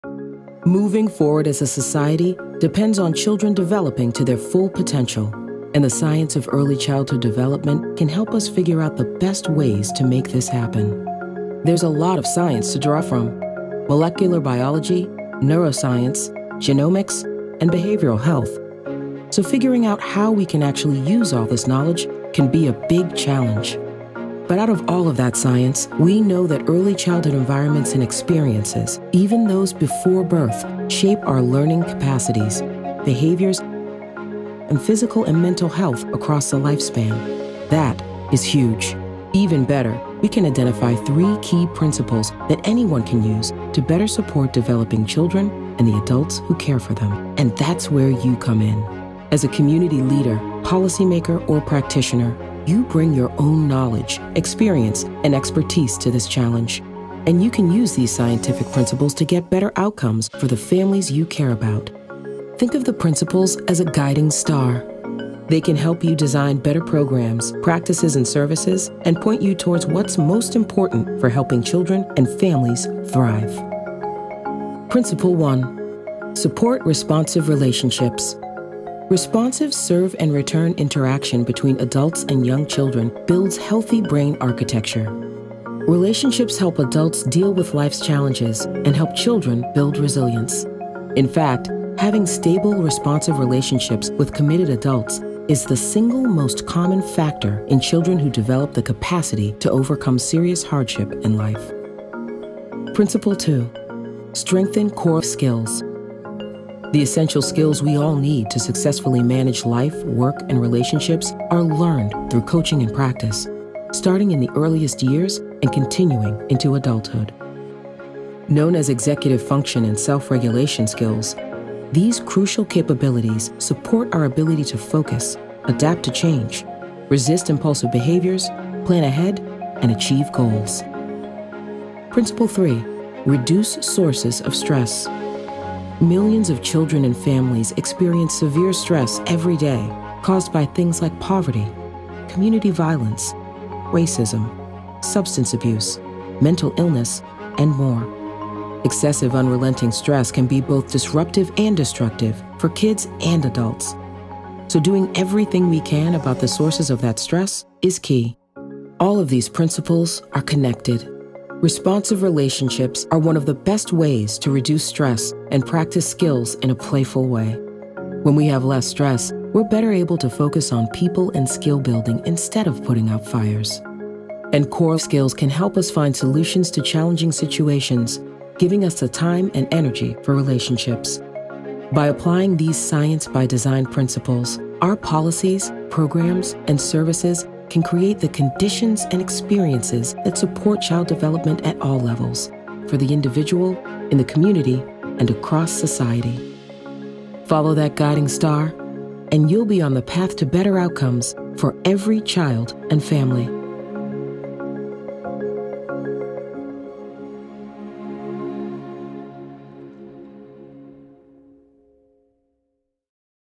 Science X Design: Three Principles to Improve Outcomes for Children
Moving forward as a society depends on children developing to their full potential, and the science of early childhood development can help us figure out the best ways to make this happen. In this video, learn about three “science by design” principles we’ve identified that community leaders, policymakers, and practitioners can use as a guiding star for designing better programs, practices, and services for children and families—ones that will help them thrive. Learn more about the Science X Design principles—and how to apply them